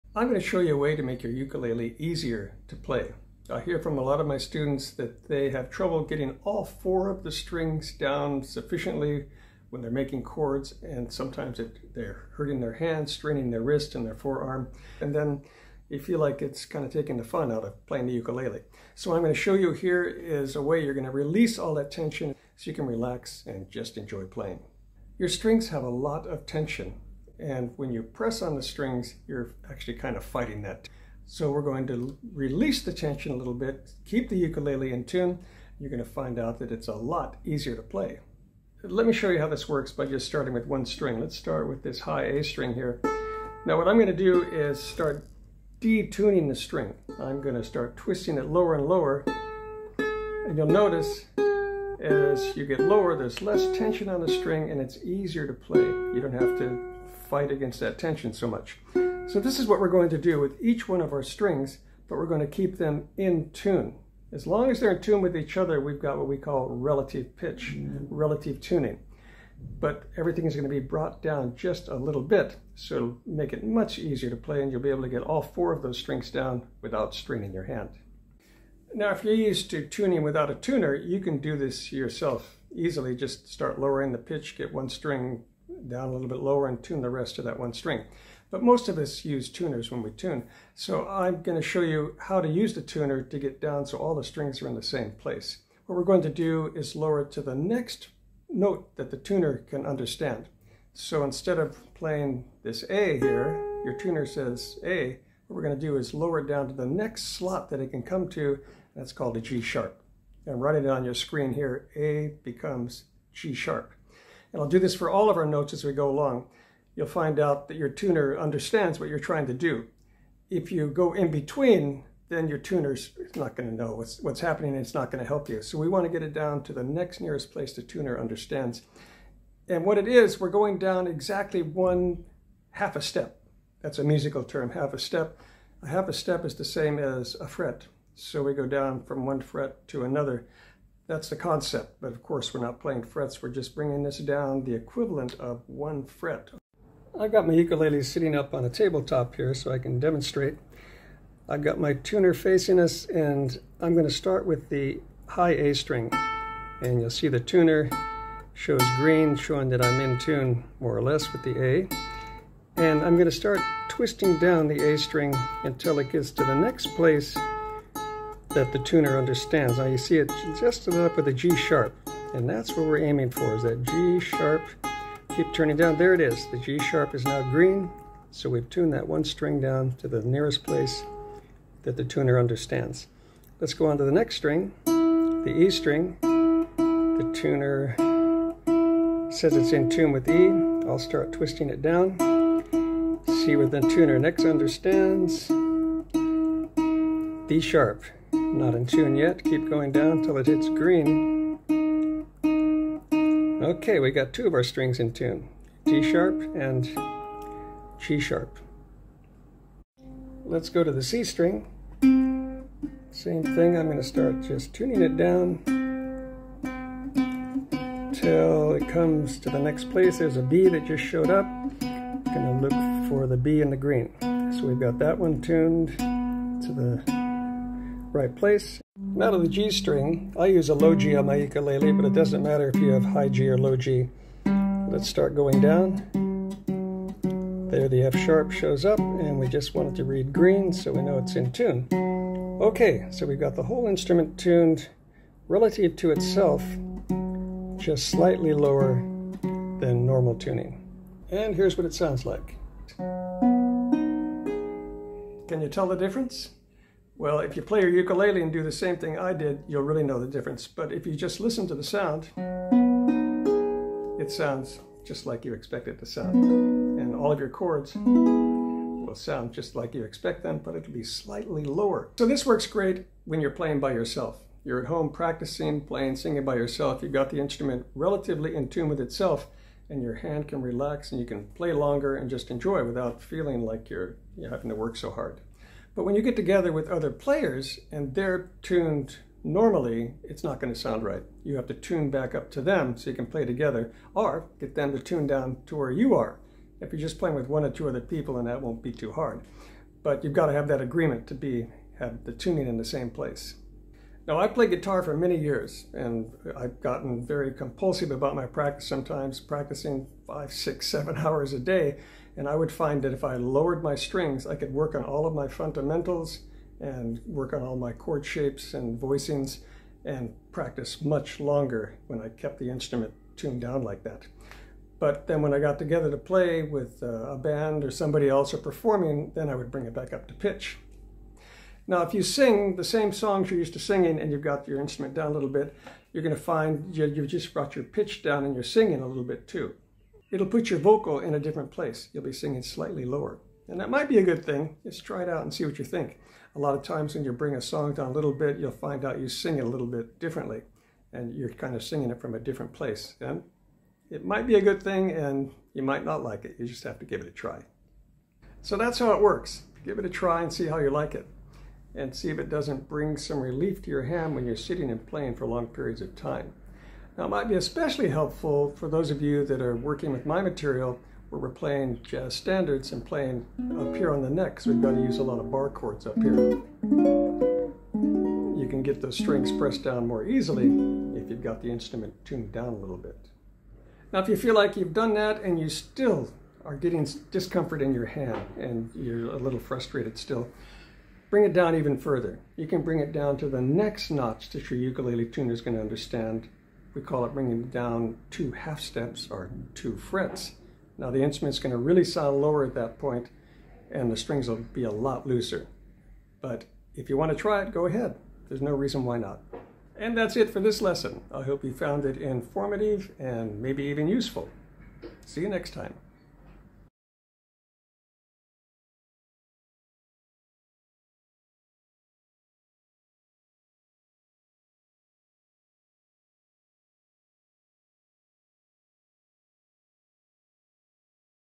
Make your ukulele easier to play - A simple trick!
Glen Rose. Play your ukulele with less hand pressure so you don’t strain and wear out your hand and wrist. Loosen your strings, but still keep it in tune with relative pitch, it will be easier to play. I show how to use your tuner to lower normal G-C-E-A tuning to F#-B-D#-G#. I have always use this tuning when I want to make my ukulele easier to play and reduce my hand fatigue.